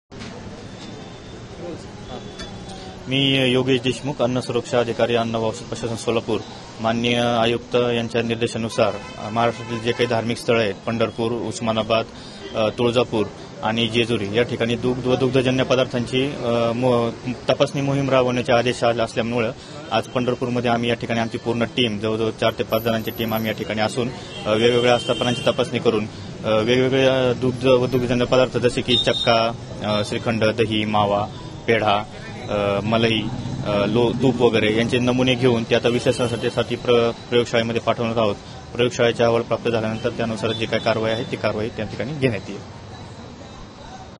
अन्न व सुरक्षा प्रशासनाकडून दुग्धजन्य पदार्थाच्या दुकानांची तपासणी
मा. आयुक्त यांच्या आदेशानुसार अन्न व सुरक्षा प्रशासन, सोलापूर यांच्या वतीने महाराष्ट्रातील धार्मिक स्थळांतील दुग्धजन्य पदार्थ विकणार्‍या दुकानांमध्ये तपासणी मोहिम सुरू करण्यात आली असल्याची माहिती अन्न सुरक्षा अधिकारी योगेश देशमुख यांनी दिली.
या धार्मिक स्थळांमध्ये पंढरपूर, उस्मानाबाद, तुळजापूर आणि जेजुरी यांचा समावेश असून 4 ते 5 अधिकारी, कर्मचारी या धार्मिक स्थळांमधील दुग्धजन्य पदार्थांच्या दुकानात जाऊन तेथील पदार्थांचे नमूने गोळा करून तपासणीसाठी प्रयोगशाळेमध्ये पाठविणार आहेत. यामध्ये दुध, चक्का, दही, खवा, पेढा आदी पदार्थांची तपासणी होणार आहे. या पदार्थांच्या तपासणीमध्ये भेसळ किंवा विषबाधाजन्य पदार्थ आढळून आल्यास अशा दुकानदारांवर योग्य ती कारवाई करण्यात येईल, असेही अन्न सुरक्षा अधिकारी देशमुख यांनी सांगितले.

पंढरी संचार  न्यूज साठी लाईक करा आणि सबस्क्राईब करा